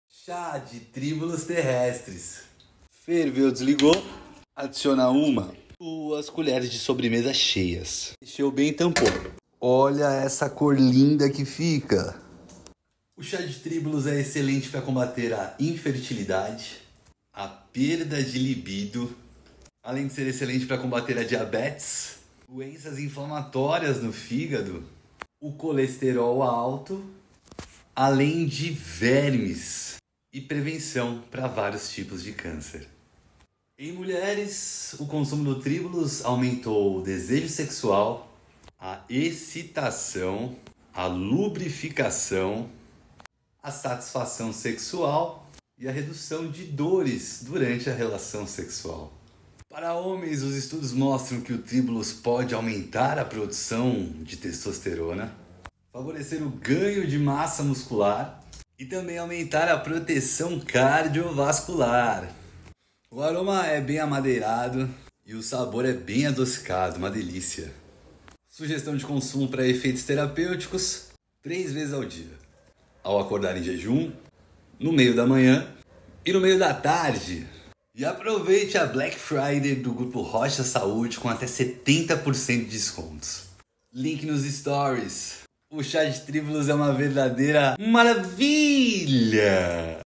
Tribullus Terrestris: Fertilidade e Libido
🌼O Tribulus terrestris é uma planta conhecida por ser usada em medicina tradicional há séculos, e o chá feito a partir dessa erva tem sido estudado por seus potenciais benefícios à saúde.
👉Excelente para:
✔️Infertilidade,
✔️Perda de libido,
✔️Diabetes,
✔️Doenças inflamatórias no fígado,
✔️Combater Vermes,
✔️Colesterol alto,
✔️Diversos tipos de câncer

👩🏻‍⚕️Nas mulheres, o consumo de TT melhorou:
✔️O desejo sexual,
✔️A excitação,
✔️A lubrificação,
✔️A satisfação sexual,
✔️A redução da dor durante a relação sexual

☑️Assim, o TT pode melhorar de forma segura e eficaz o desejo em mulheres com transtorno de desejo sexual hipoativo.

👨🏻‍⚕️Nos homens o TT:
▪️↑ produção de testosterona em homens saudáveis
▪️↑ produção de hormônio luteinizante (LH)
▪️↑ crescimento muscular.
▪️↓ inflamação
▪️↓ dano oxidativo no músculo,
▪️↑ proteção cardiovascular

📚Os principais compostos bioativos são: flavonóides, alcalóides, saponinas, lignina, amidas e glicosídeos. Com destaque para furostanol e espirostanol saponósido, considerados os princípios ativos relacionados com as ações terapêuticas.

🫖Chá de Tribullus 🌼
▪️2 col de sobremesa de flores secas de TT

👨🏻‍⚕️Preparo:
1️⃣Ferva a água
2️⃣Desligue e adicione o TT. Mexa bem e tampe por 15 min.
3️⃣Coe e consuma

👉Sugestão: 3 xícaras ao dia.
✔️Ao acordar em jejum
✔️No meio da manhã
✔️No meio da tarde

🫶🏻Se você já conhecia a erva TT, escreva “sim” nos comentários❤️

📚Akhtari, E. et al. Tribulus terrestris for treatment of sexual dysfunction in women: randomized double-blind placebo - controlled study. Daru, 2014.
📚Ștefănescu, R. et al. A Comprehensive Review of the Phytochemical, Pharmacological, and Toxicological Properties of Tribulus terrestris L. Biomolecules, 2020.
📚Ghanbari, A. et al. Tribulus terrestris and female reproductive system health: A comprehensive review. Phytomedicine, 2021.
📚Phytopharmacology of Tribulus terrestris
M Shahid et al. J Biol Regul Homeost Agents. 2016.